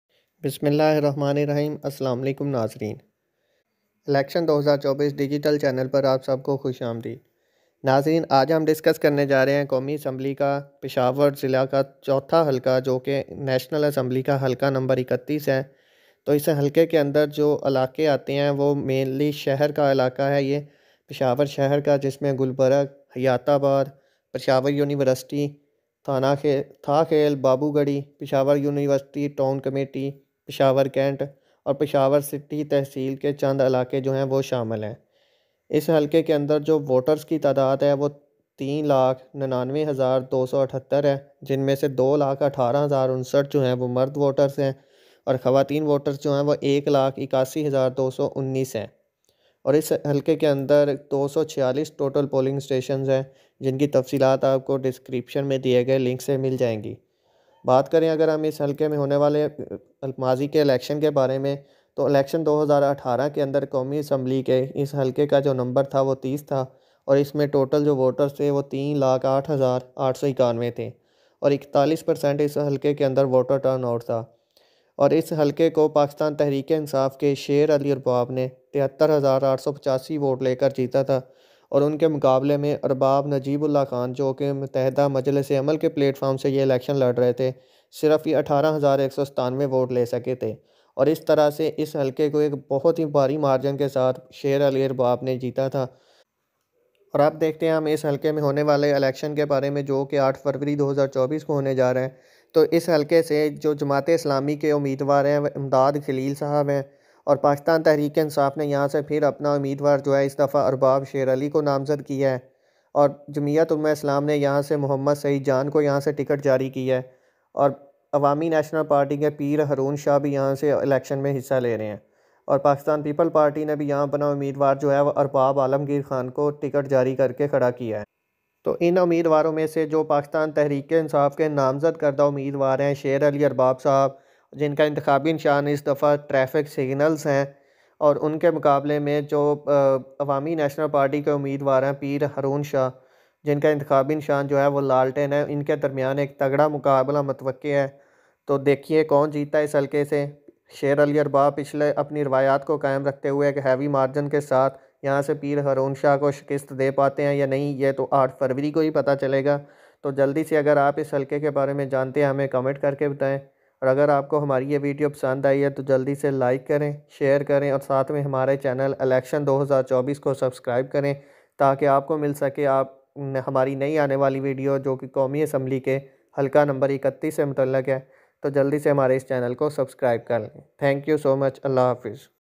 PAKISTAN Election 2024 NA 31 / Peshawar PTI vs ANP / Arbab Sher Ali vs Peer Haroon Shah
This video is related to all information about National assembly halqa no NA 31. This seat is belongs to District Peshawar kpk. Its about pti vs Awami national party candidates.
its all about halqa no NA 31#pakistan